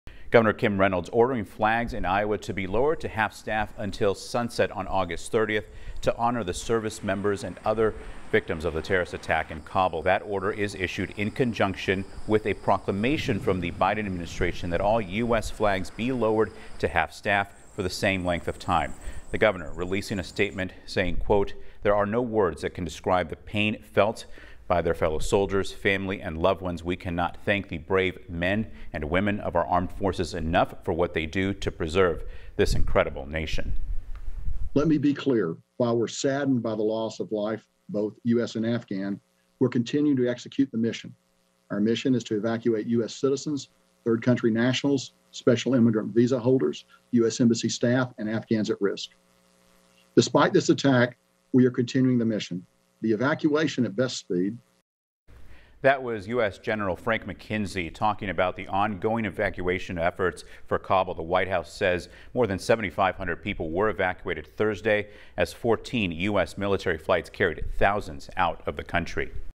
Gov. Reynolds orders flags to fly half-staff in honor of US service members, Afghans killed in Kabul
As of Friday morning, the death toll from the attack at the Hamid Karzai International Airport has surpassed 100. At least 95 Afghans and 13 U.S. troops were killed.